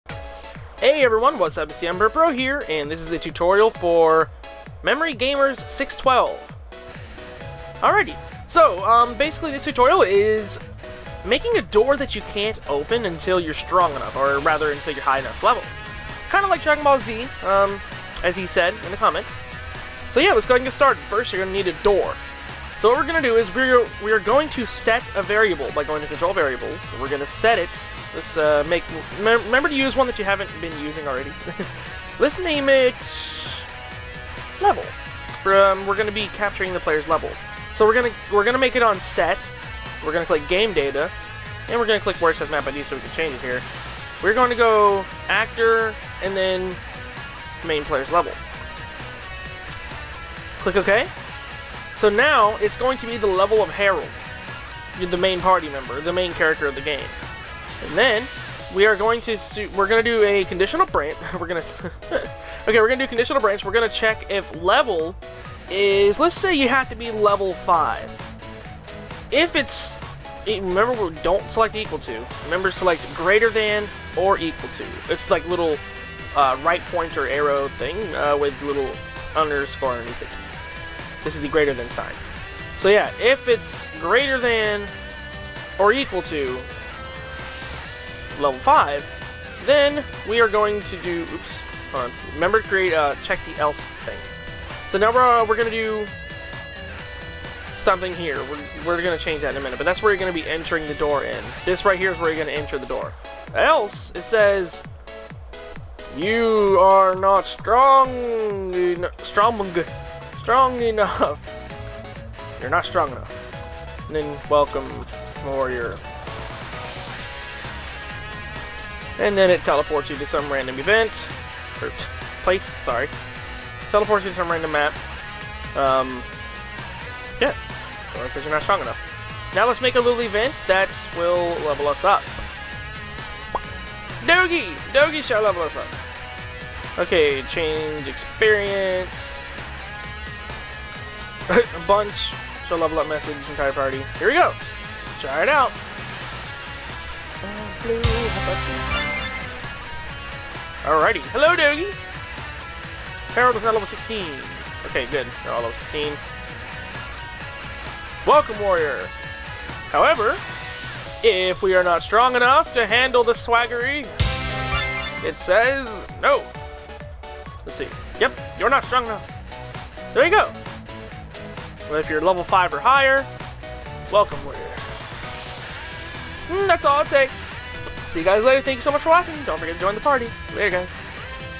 RPG Maker MV: Tutorial for MemoryGamers612 - Open door if level is high enough
HI guys! This is a tutorial for MemoryGamers612. How to open a door if your level is high enough :D

Want to be a YouTube partner? See if you qualify for MakerGen here!